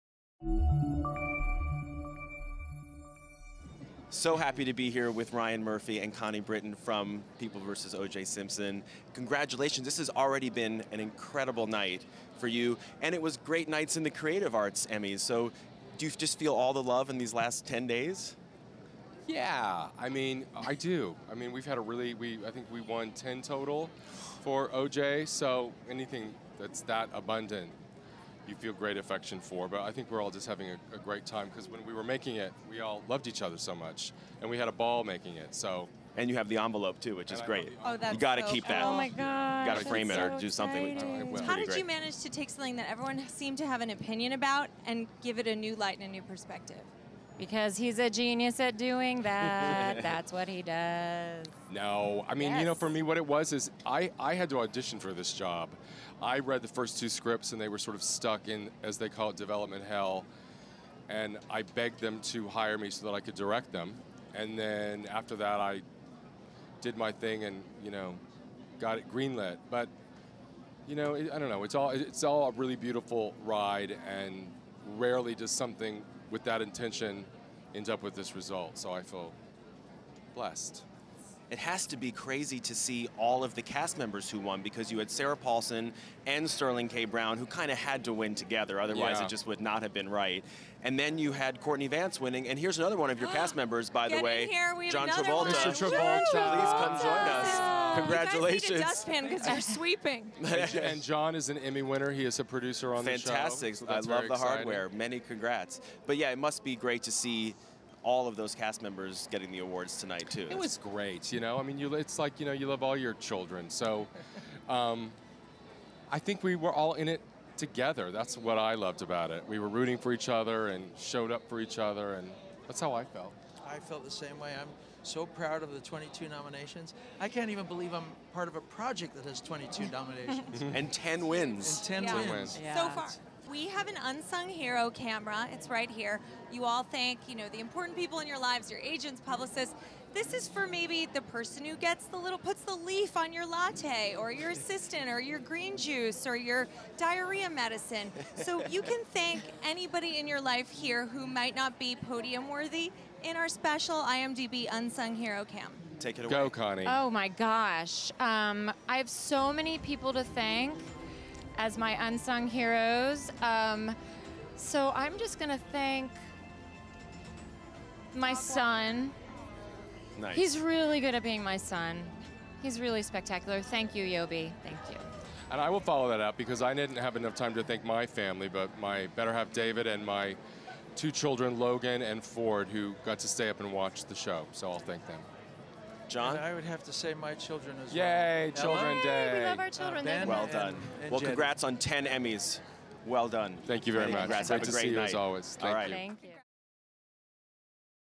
John Travolta, Connie Britton, and Ryan Murphy on "The People v. O.J. Simpson"
"The People v. O.J. Simpson: American Crime Story" took home the trophy for Outstanding Limited Series at the 2016 Emmys. Producer Ryan Murphy and co-stars John Travolta and Connie Britton sat down with "IMDb LIVE After the Emmys" hosts Dave Karger and Jill Kargman to talk about their big win. The show won a total of nine Emmys in 2016.